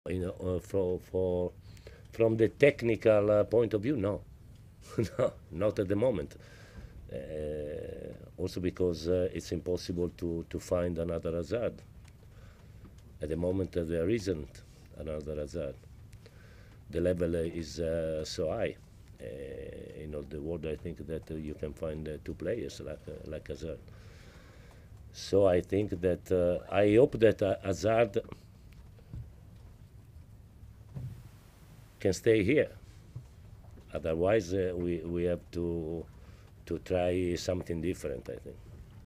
Sarri: "No nos podemos permitir la salida de Hazard" | Diario AS
El técnico del Chelsea destacó la relevancia del belga en la plantilla: "Es imposible encontrar otro Hazard" e hizo hincapié en su deseo de que se quede.